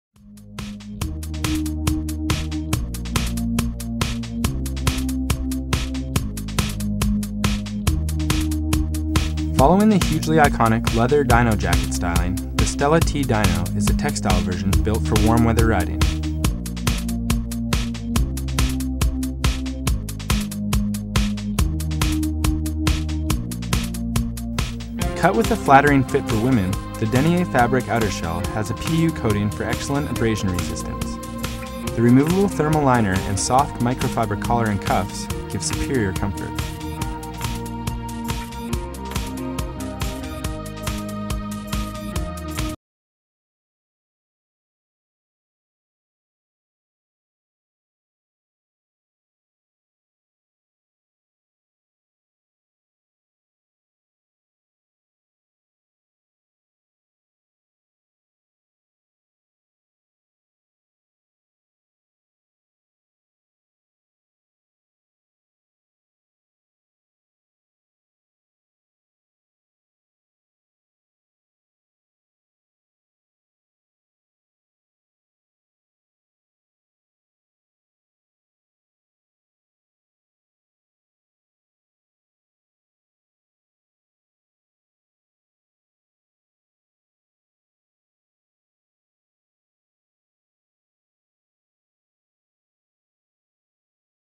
Alpinestars - Jaqueta Feminina Stella T-Dyno (legendado)
Video de apresentação da jaqueta feminina Stella T-Dyno legendado.